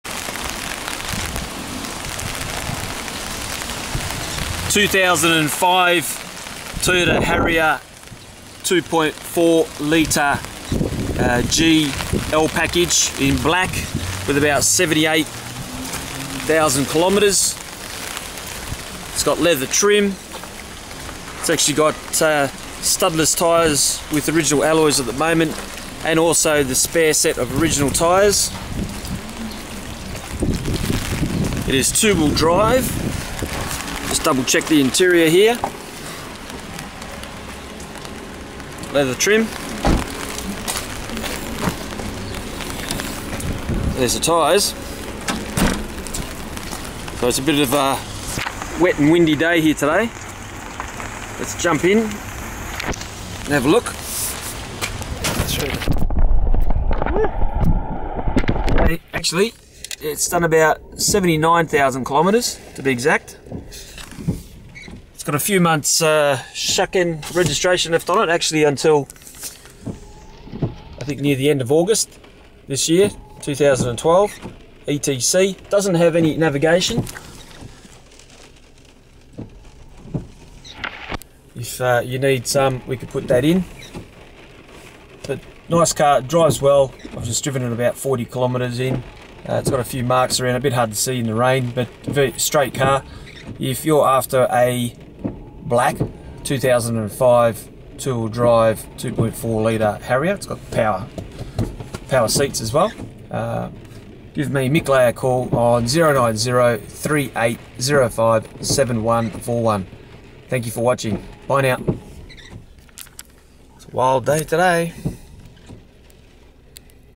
2005 Toyota Harrier G L Package
Call Tokyo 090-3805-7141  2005 Toyota harrier with leather.  2.4ltr, auto transmission, front power seats, about 79,900km, Call Mick Lay for details on this car.  Lease or cash offers looked at.